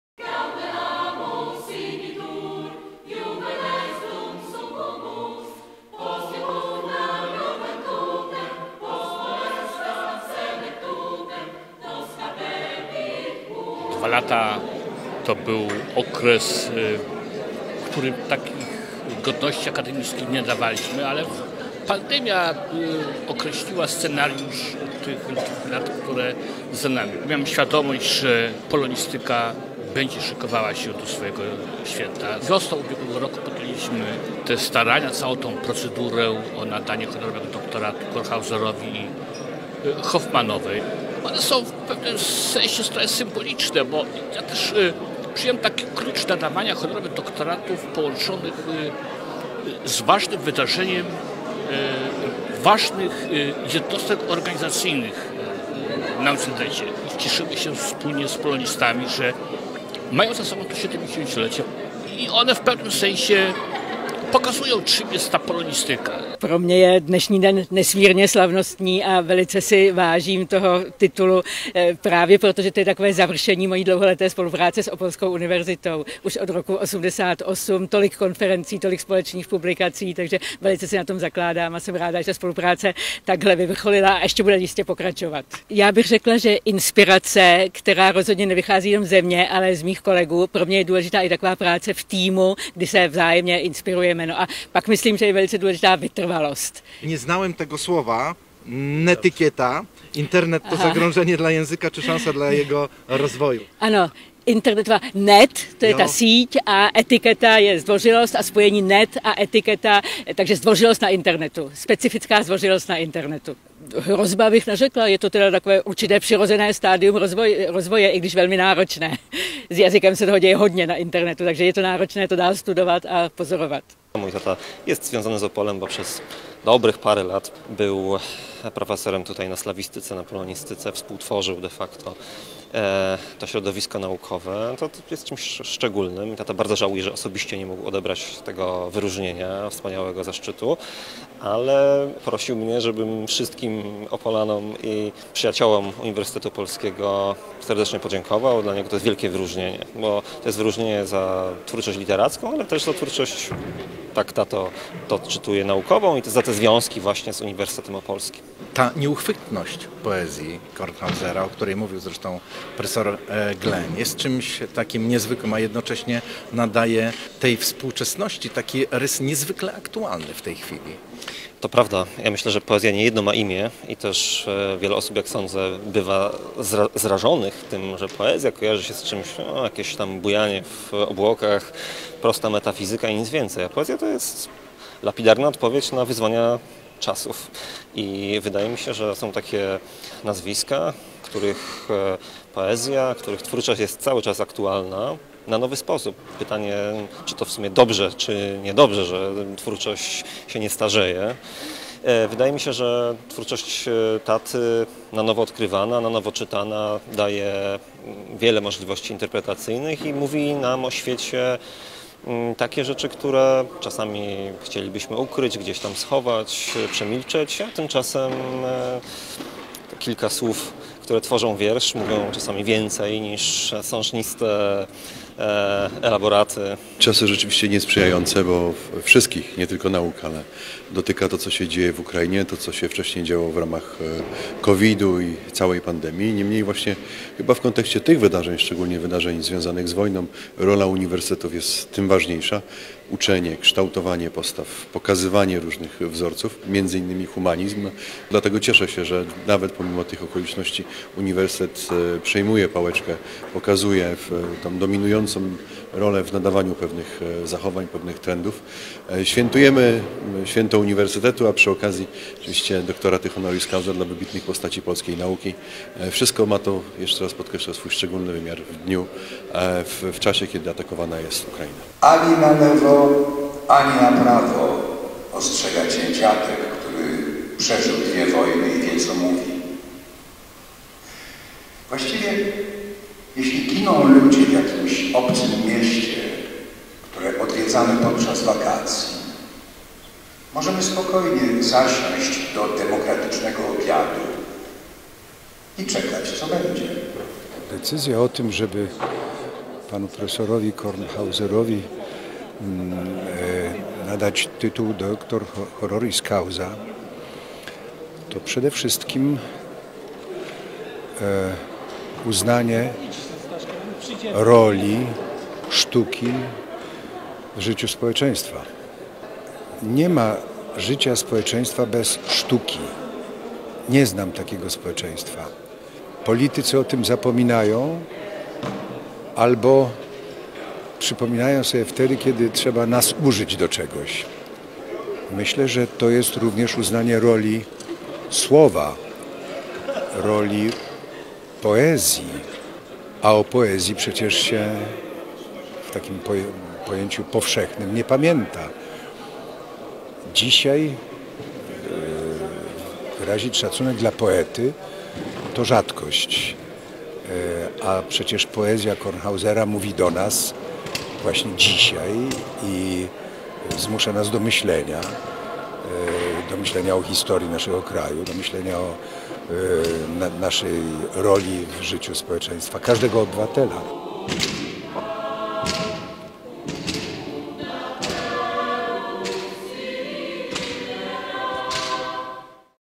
Święto Uniwersytetu Opolskiego 2022 - nadanie tytułów Doctor Honoris Causa
Dokładnie po dwóch latach Uniwersytet Opolski przyznał tytuły Doctor Honoris Causa. Uroczystość odbyła się w ramach tegorocznego święta tej największej w Opolu uczelni.